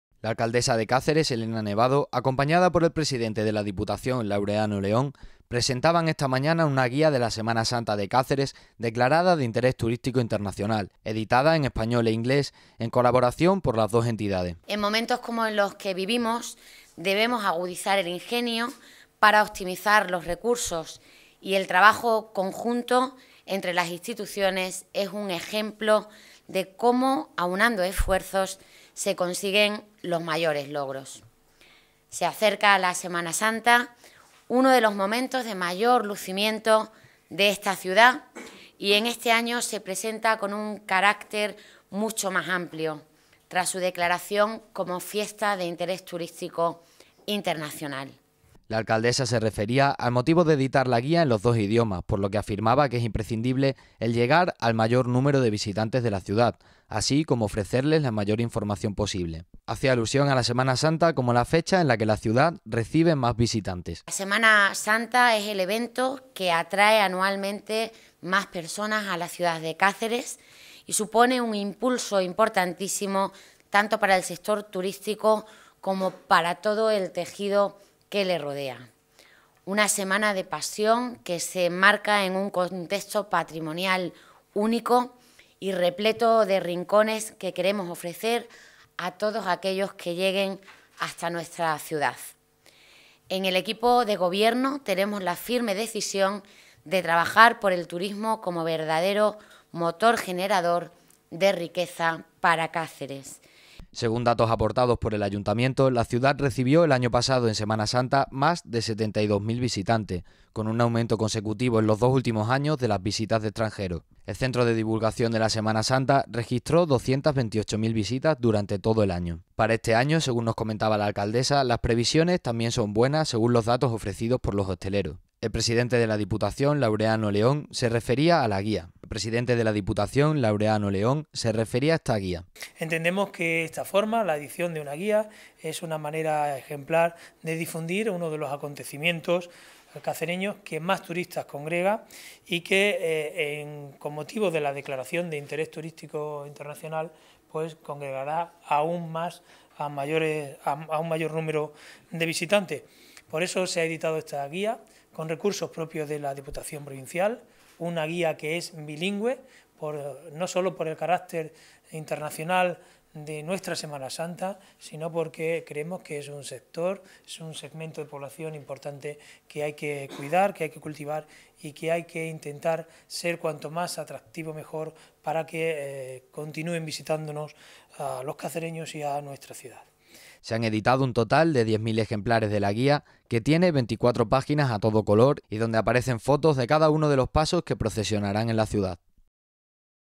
Ayuntamiento y Diputación de Cáceres, editan una guía bilingüe de la Semana Santa.
La Alcaldesa de Cáceres, Elena Nevado, acompañada por el Presidente de la Diputación, Laureano León, presentaban esta mañana, una guía de la Semana Santa de Cáceres, declarada de interés turístico Internacional, editada en español e inglés, en colaboración por las dos entidades.

La Alcaldesa se refería al motivo de editar la guía en los dos idiomas, por lo que afirmaba que es imprescindible el llegar a el mayor número de visitantes de la ciudad, así como ofrecerles la mayor información posible.

Hacía alusión a la Semana Santa, como la fecha en la que la ciudad recibe más visitantes.

Según datos aportados por el Ayuntamiento, la ciudad recibió el año pasado en semana santa, más de 72.000 visitantes, con un aumento consecutivo en los dos últimos años de las visitas de extranjeros.

El centro de Divulgación de la Semana Santa, registró 228.000 visitas durante todo el año.

Para este año, según nos comentaba la Alcaldesa, las previsiones también son buenas, según los datos ofrecidos por los hosteleros.

El Presidente de la Diputación, Laureano León, se refería a la guía.

Se han editado un total de 10.000 ejemplares de la guía, que tiene 24 paginas a todo color y donde aparecen fotos de cada uno de los pasos que posesionarán en la ciudad.

*k30caceres* *ayuntamiento* *cultura*